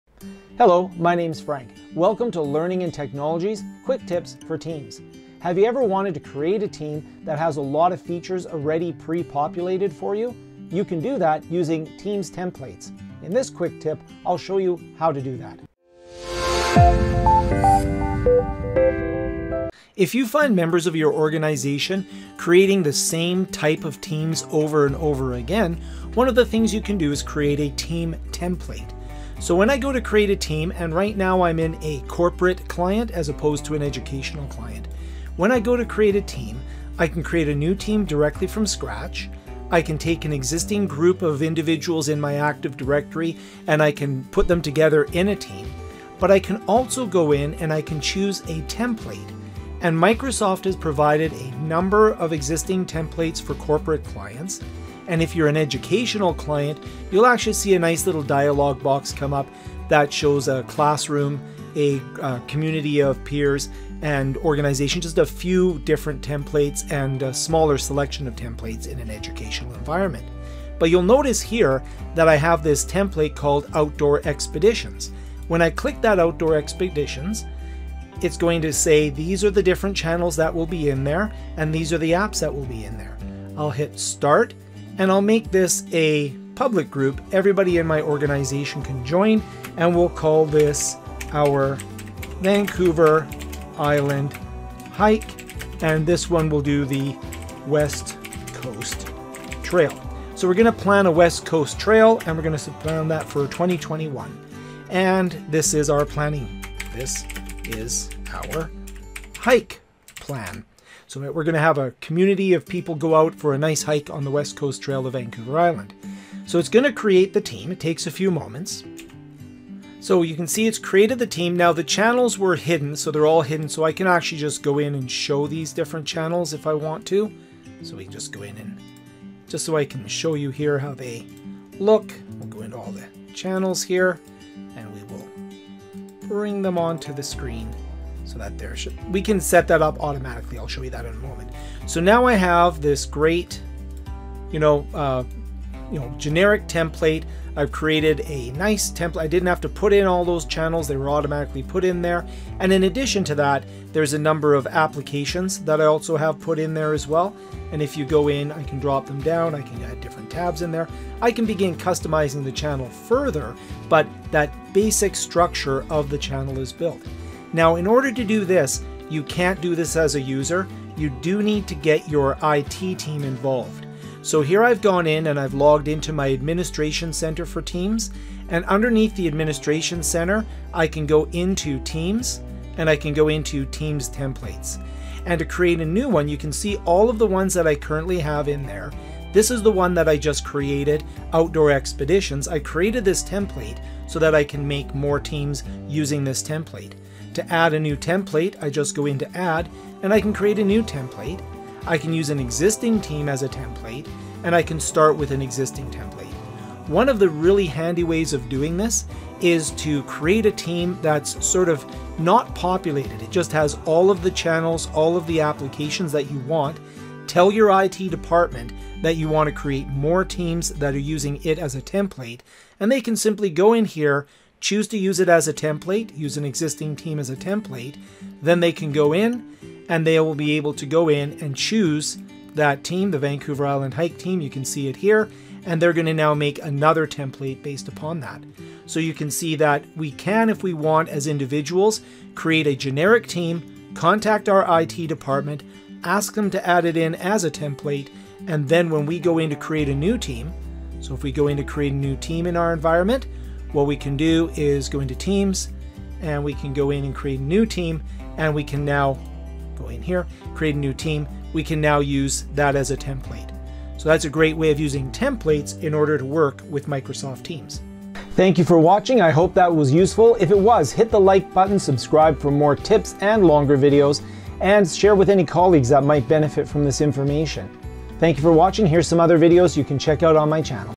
How to Create and Use Templates in Microsoft Teams - Quick Tip #11
Templates are a great way to help your users create a Team that is "pre-built" for a specific purpose: Sales, Social, Projects, and more!